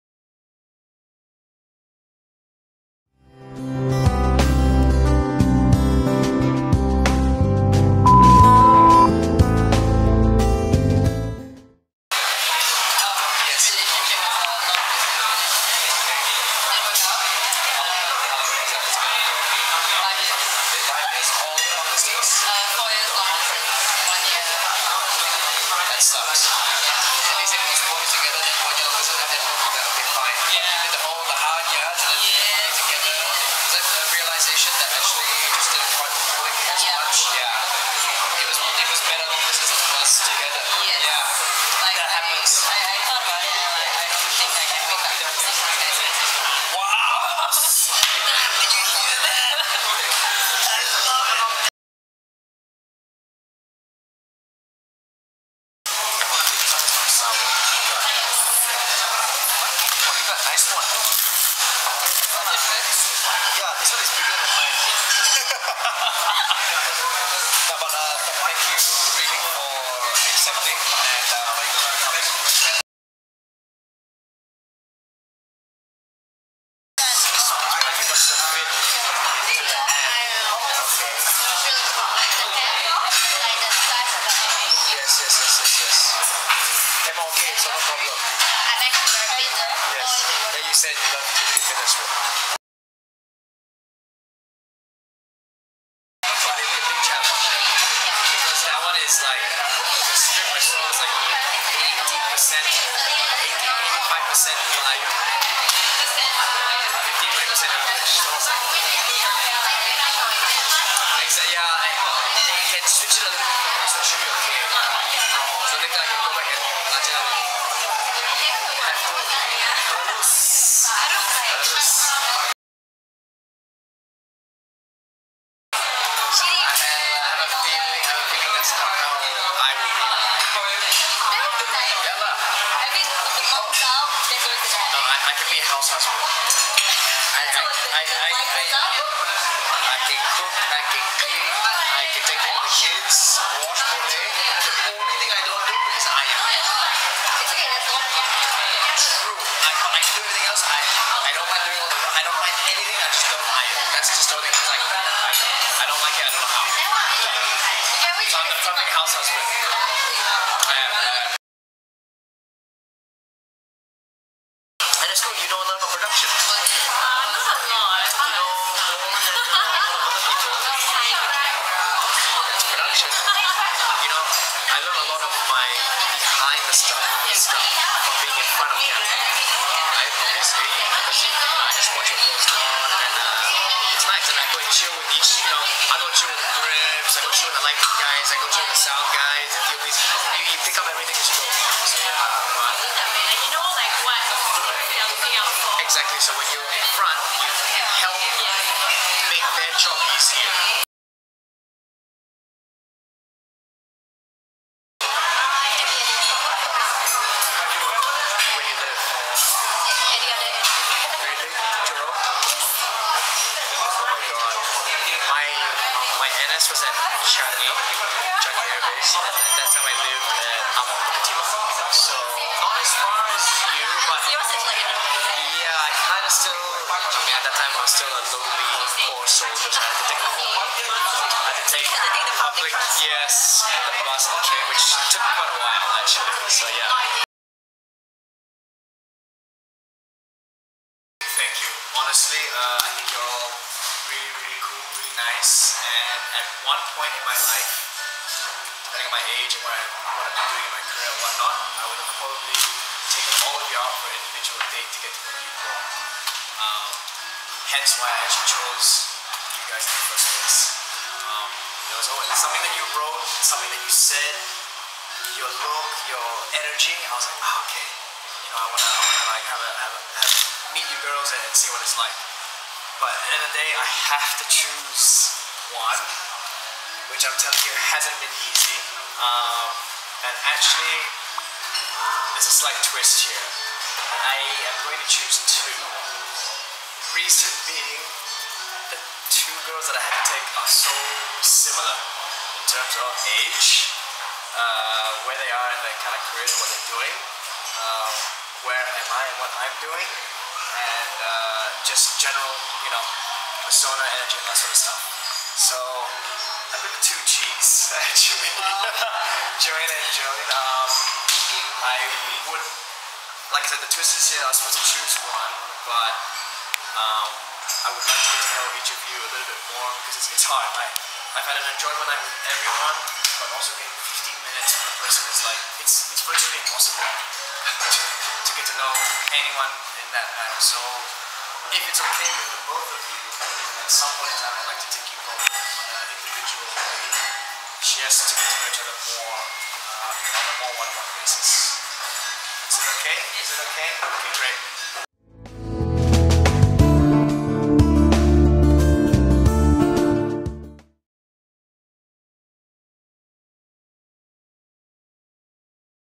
Paul Foster finds his dates
Paul Foster on a group date at Resorts World Sentosa where he gets to know them a little better. Watch to see who he chooses.